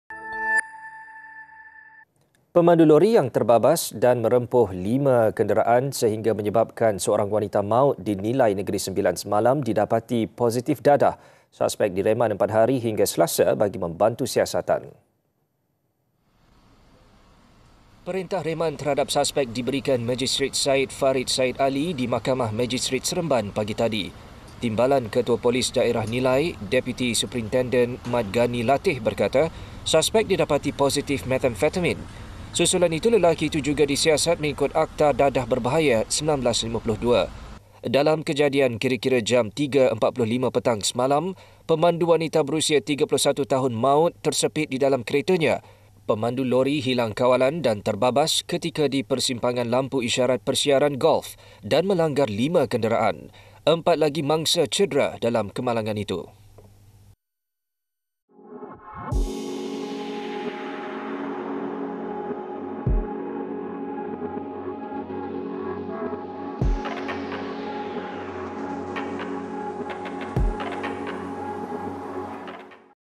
KEMALANGAN | Pemandu Lori Rempuh Lima Kenderaan Positif Dadah
Pemandu lori yang terbabas dan merempuh lima kenderaan sehingga menyebabkan kematian seorang wanita di Nilai, Negeri Sembilan semalam, didapati positif dadah.